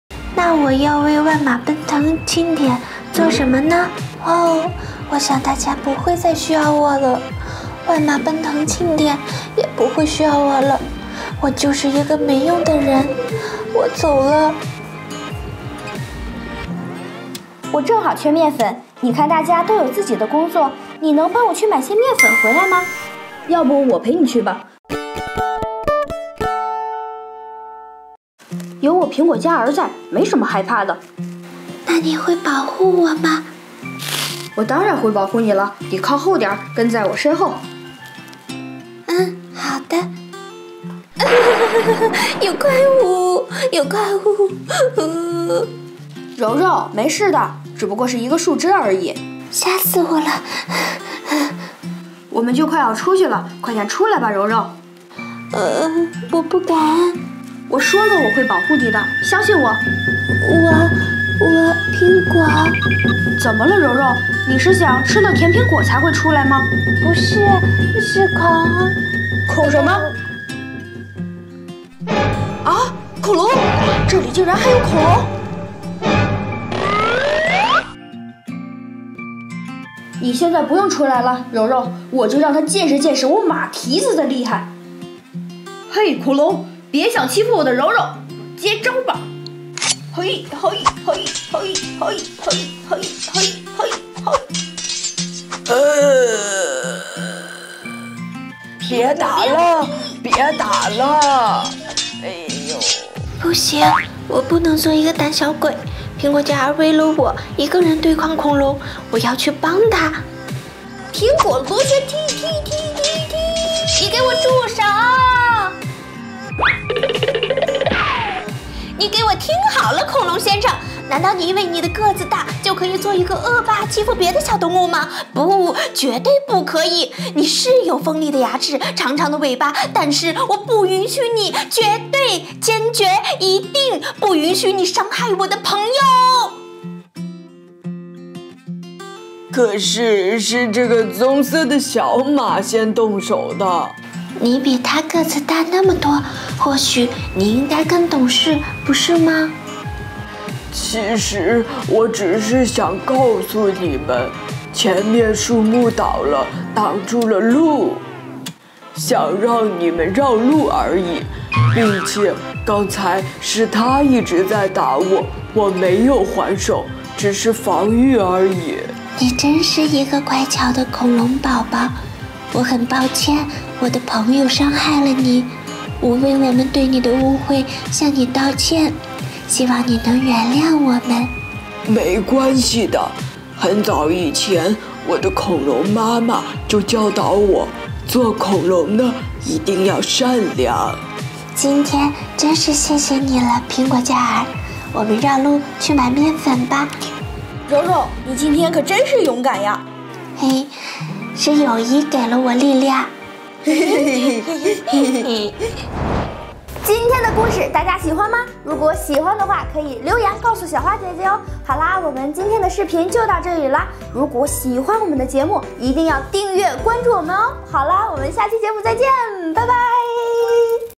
玩具故事：柔柔勇敢点！之柔柔竟然驯化了恐龙
在去执行任务的路上，苹果嘉儿和柔柔碰到了恐龙，接下来会发生什么事情呢？